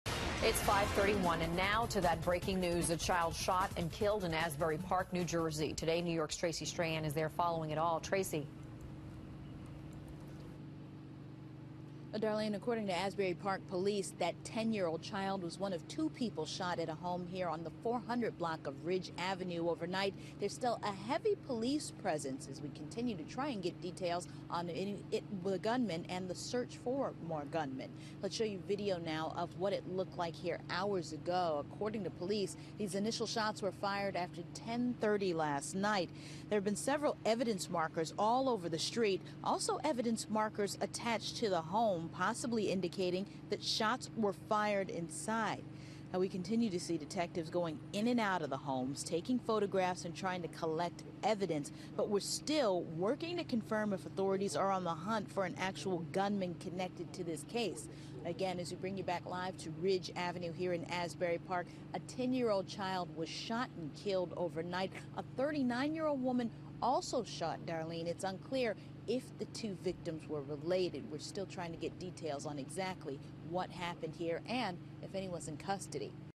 10-Year-Old Child Shot to Death in New Jersey, Woman Wounded: Officials
A 10-year-old child was shot to death in New Jersey late Wednesday, authorities say.

An official also said a 39-year-old woman was shot in the leg after the bullets went flying in Asbury Park around 10:30 p.m. She is still alive, according to the official, however, her condition was not known.

Video from the scene shows several evidence markers on the street in front of a home on Ridge Avenue.

It wasn’t clear if either victim was targeted or what may have led up to the shooting. There was no immediate word if anybody has been arrested.